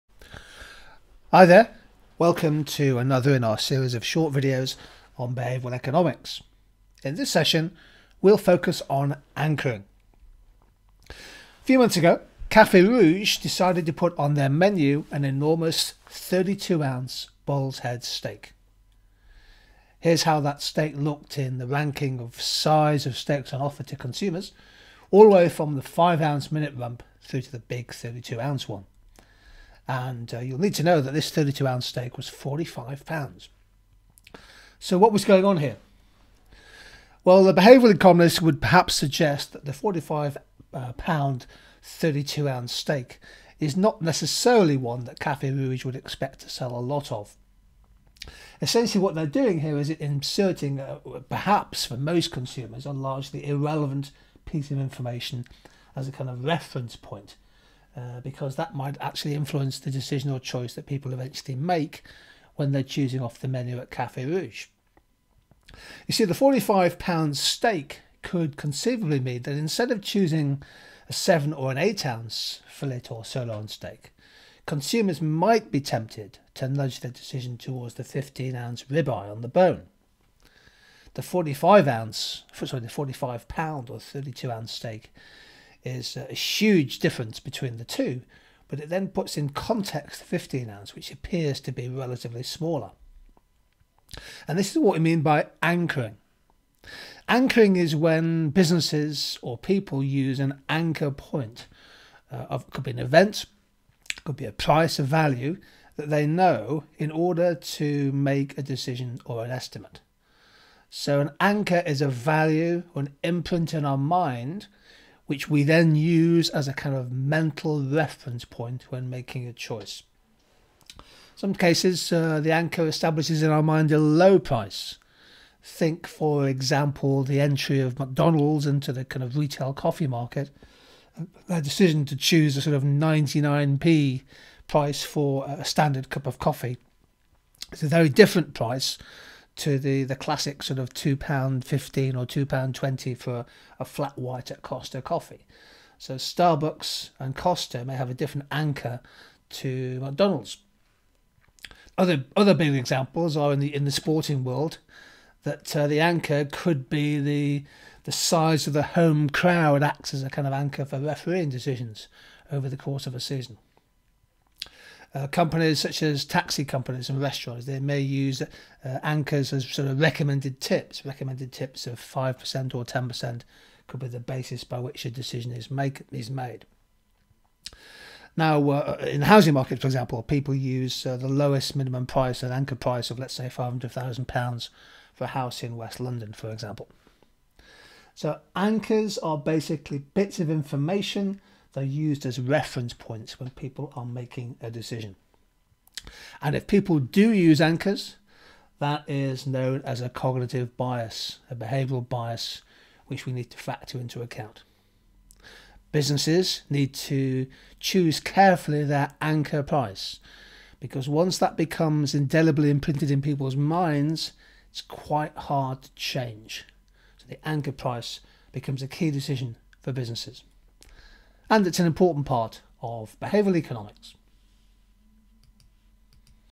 Behavioural Economics - Anchoring
Anchoring is the use of (usually) irrelevant information as a reference point for helping to make an estimate of an unknown piece of information. In other words, people use an “anchor point” of an event or a value that they know in order to make a decision or estimate. Behavioural scientists describe this as a cognitive bias.
MORE ABOUT TUTOR2U ECONOMICS:

Visit tutor2u Economics for thousands of free study notes, videos, quizzes and more: